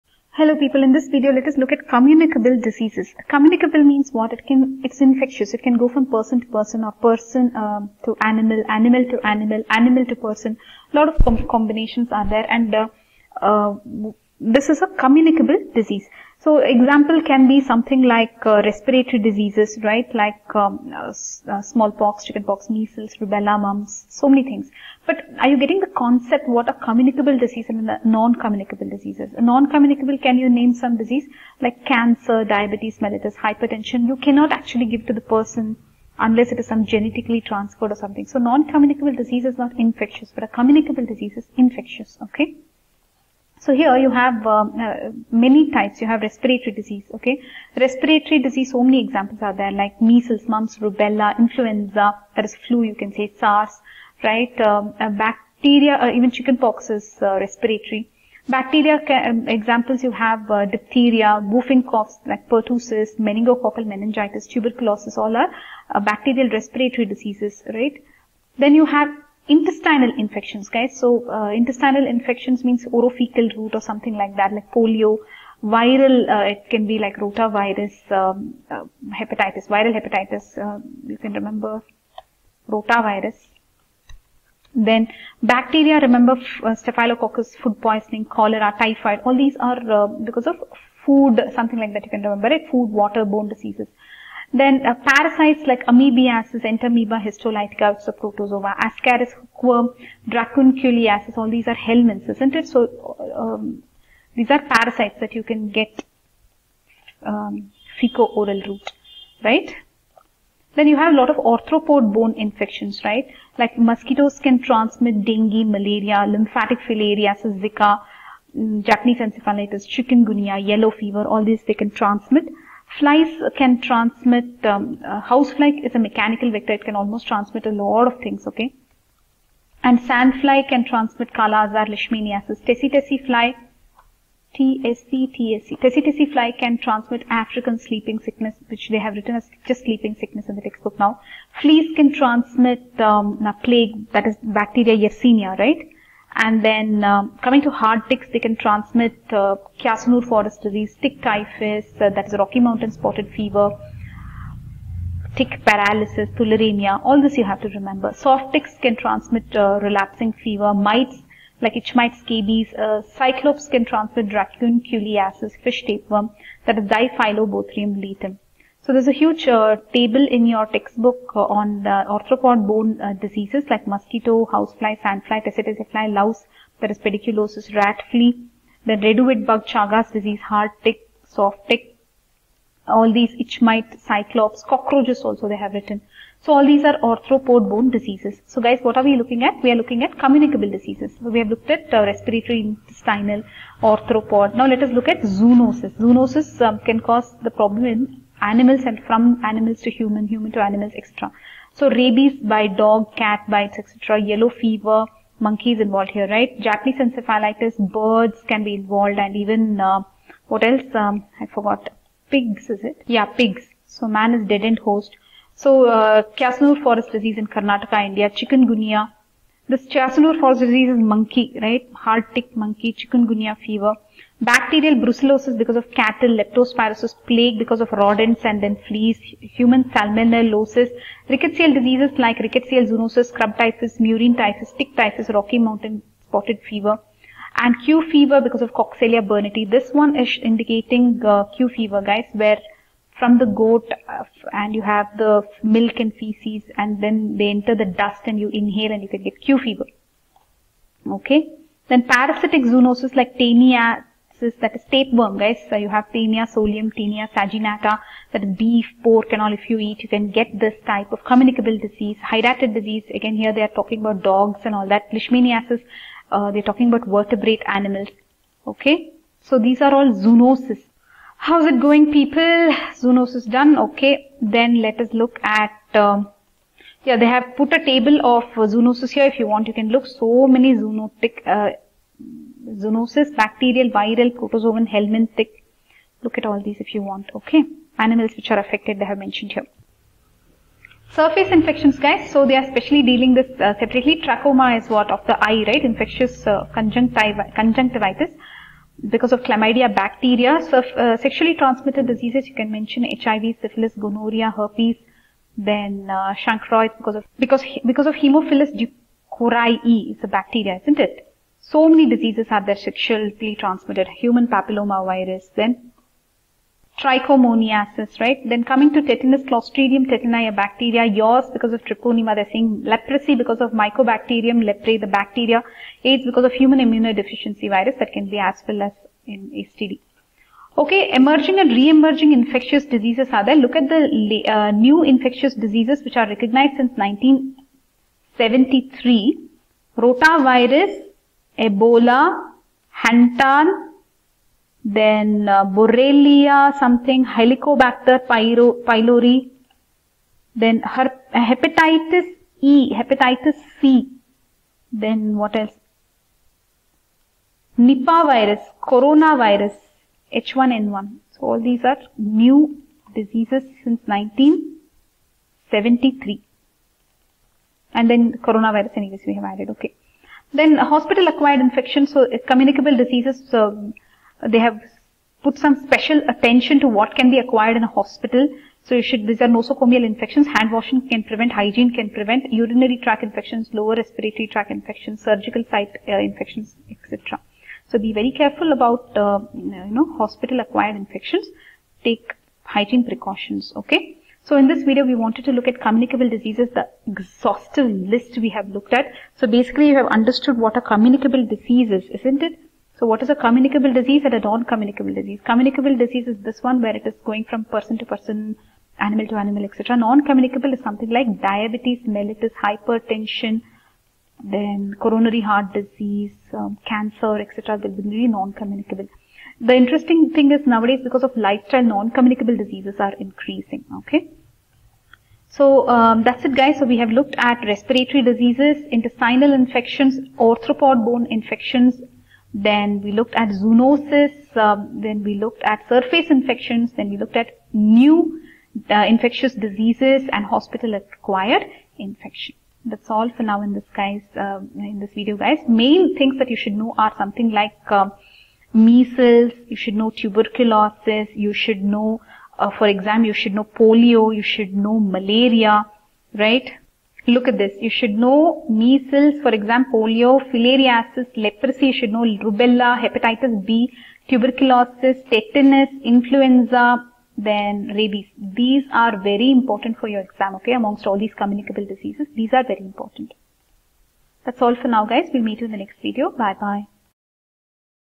PSM 153 Communicable Diseases Arthropod Infections Infectious List Vector Zoonoses Examples
0:45 Respiratory
1:08 Intestinal
1:50 Arthropod Borne Diseases
3:12 Zoonoses
4:50 Surface, STDs
5:30 New Infectious diseases
6:10 Hospital Nosocomial Infections
6:33 Revision